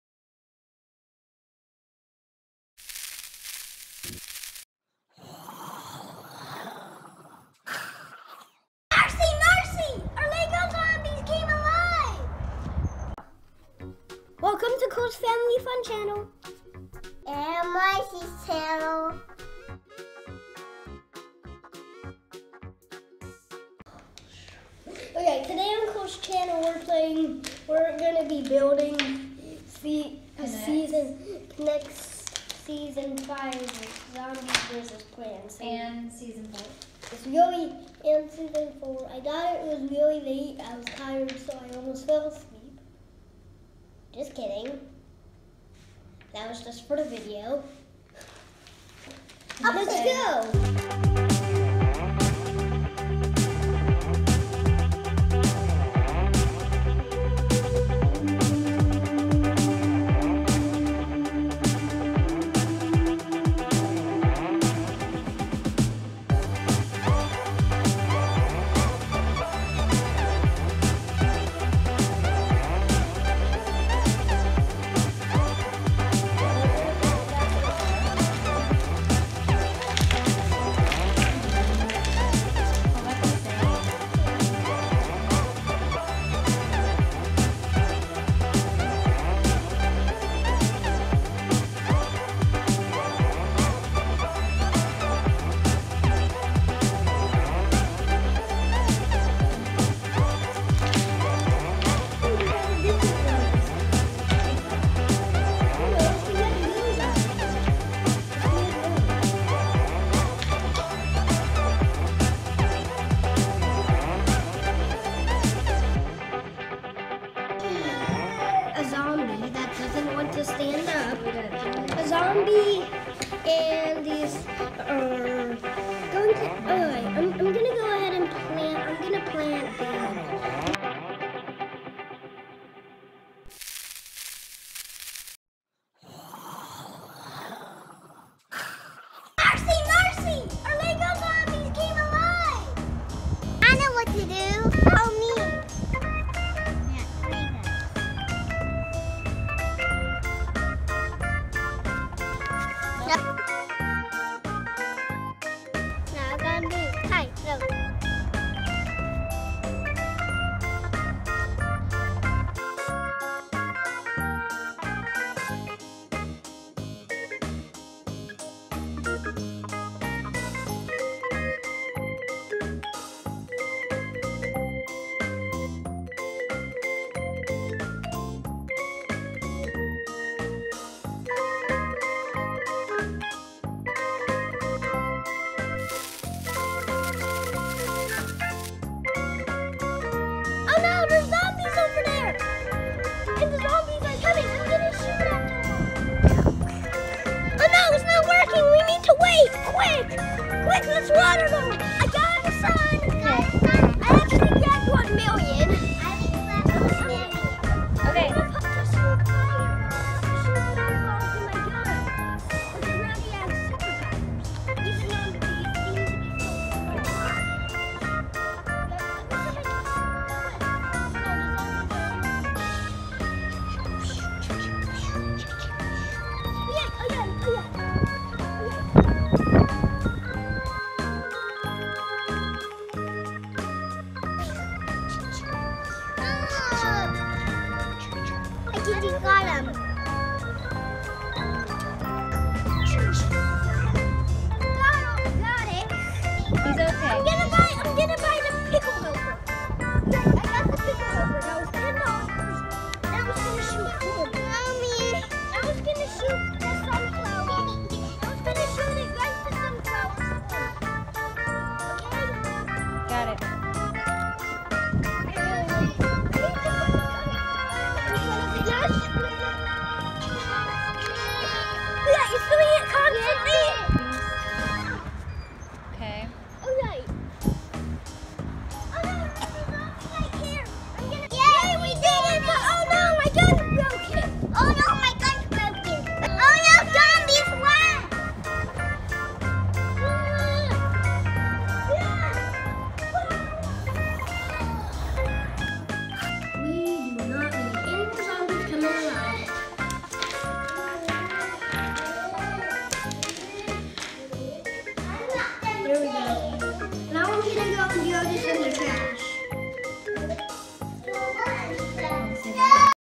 Plants vs. Zombies In Real LIfe ! | K'NEX blind Bags series 4 and 5
Cole and marci Open series 4 and 5 k'nex plants vs. zombies blind bags.
this is just a skit we made for fun :)